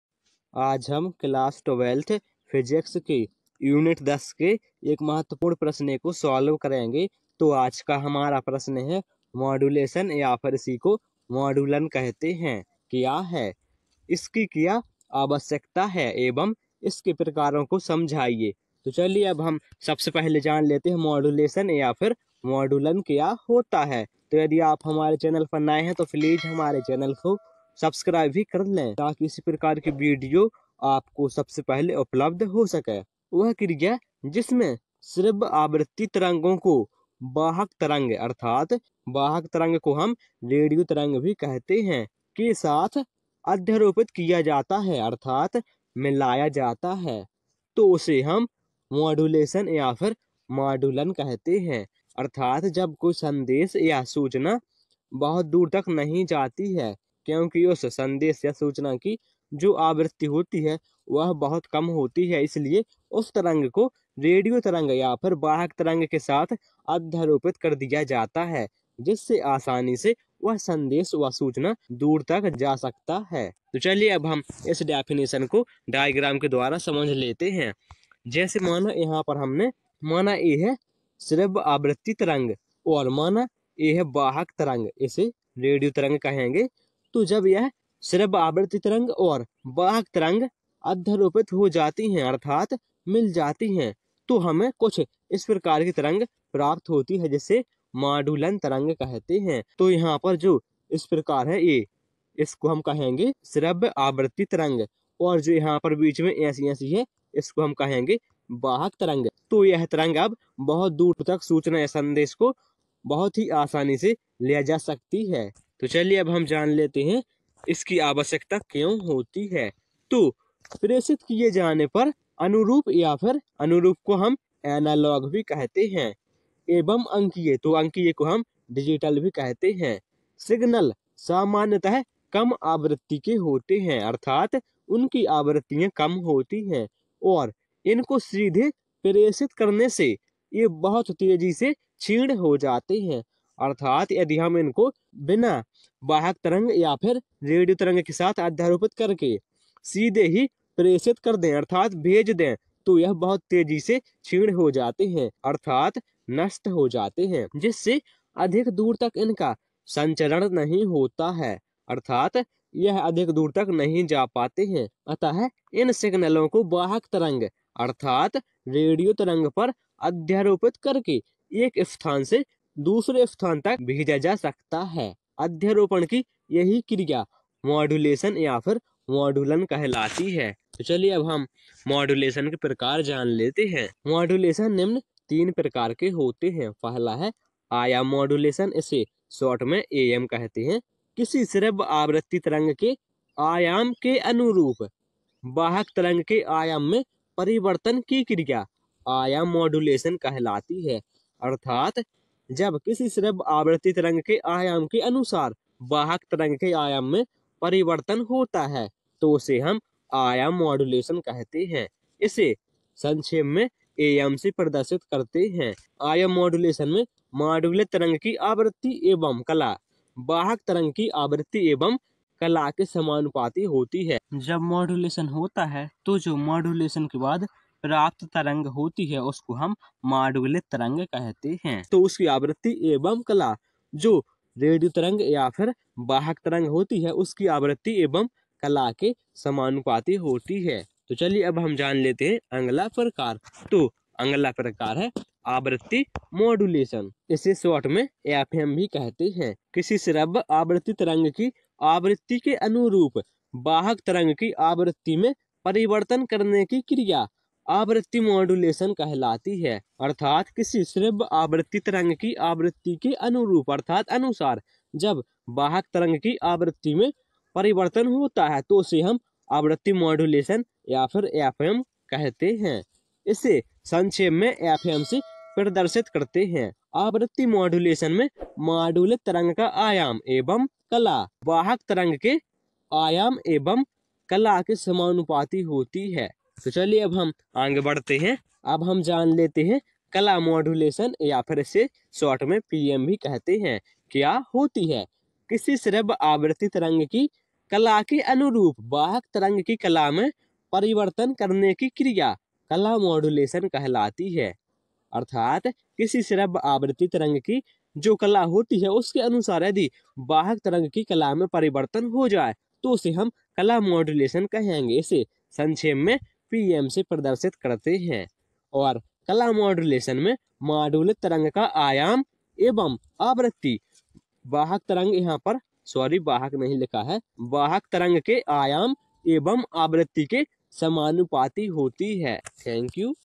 🔥माड्यूलेशन (मॉड्यूलन) क्या है?|एवं इसकी आवश्यकता क्यों है|एवं इसके प्रकार🔥class-12th physics
Hi Friends-